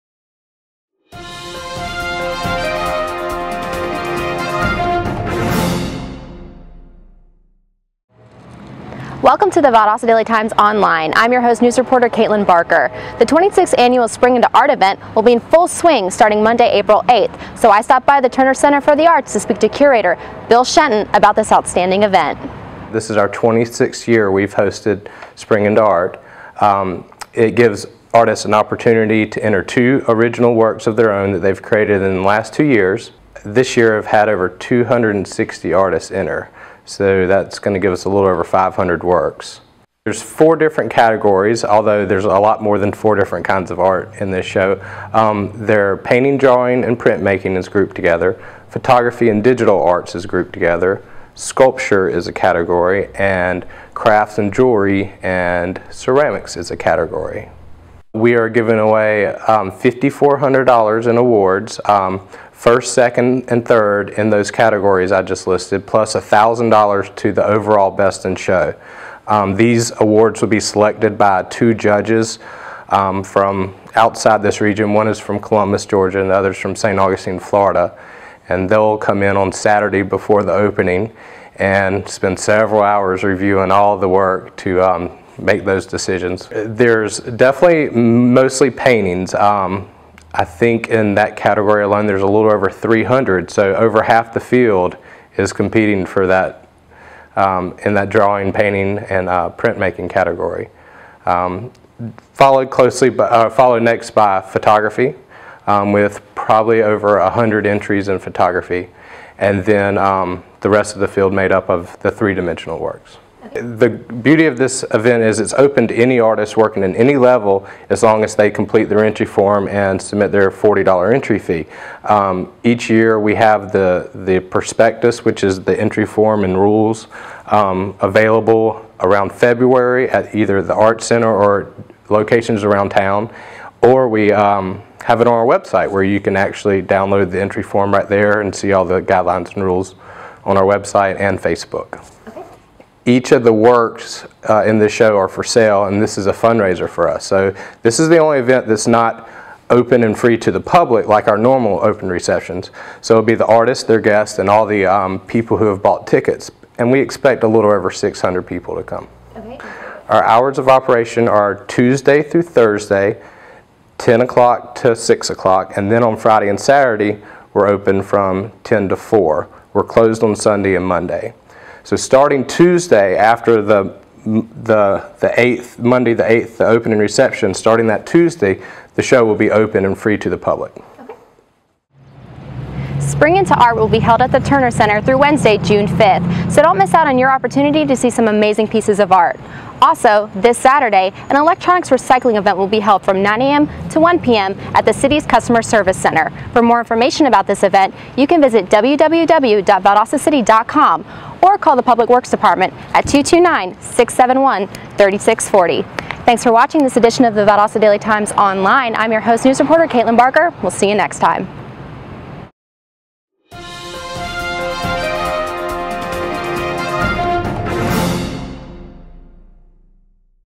Spring into Art
News Reporter Caitlin Barker speaks to Bill Shenton, Curator for the Annette Howell Turner Center for the Arts, about the Spring into Art Exhibit which will begin Monday April 8th-Wednesday June 5th. Also, the Valdosta Public Works Department will host an Electronics Recycling Event this Saturday from 9 a.m. to 1 p.m. in the parking lot of the customer service center on North Lee Street.